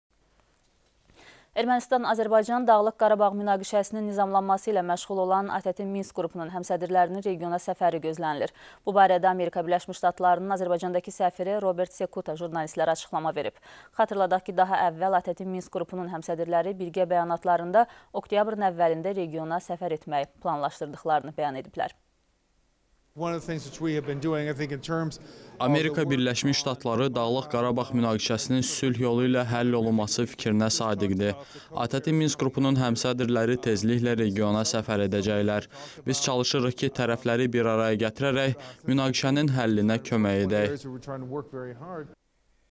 HƏMSƏDRLƏR REGİONA SƏFƏR EDƏCƏKLƏR
Ermənistan-Azərbaycan, Dağlıq Qarabağ münaqişəsinin nizamlanması ilə məşğul olan ATƏT-in Minsk qrupu həmsədrlərinin regiona səfəri gözlənilir. Bu barədə Amerika Birləşmiş Ştatlarının Azərbaycandakı səfiri Robert Sekuta jurnalistlərə açıqlama verib. Xatırladaq ki, daha əvvəl ATƏT-in Minsk qrupunun həmsədrləri birgə bəyanatlarında oktyabrın əvvəlində regiona səfər etməyi planlaşdırdıqlarını bəyan ediblər.